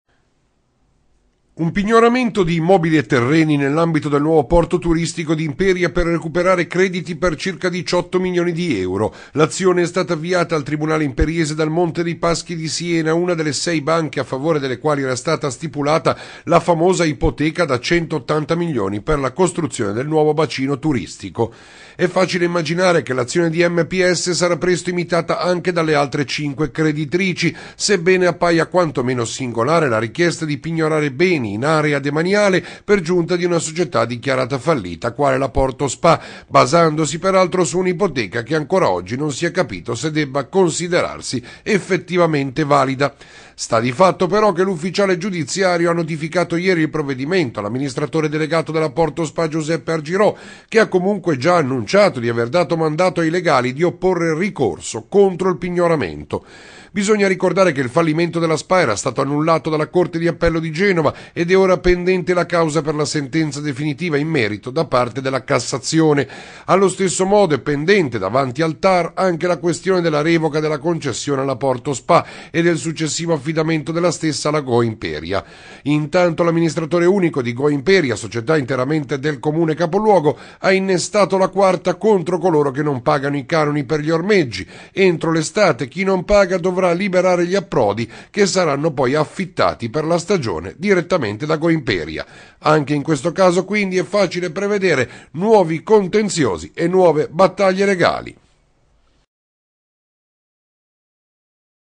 PORTO DI IMPERIA : CHIESTO IL PIGNORAMENTO
LA BANCA MONTE DEI PASCHI DI SIENA CHIEDE IL PIGNORAMENTO DEI BENI DEL NUOVO BACINO TURISTICO PER RECUPERARE UN CREDITO DI 18 MILIONI DI EURO DALLA PORTO DI IMPERIA SPA.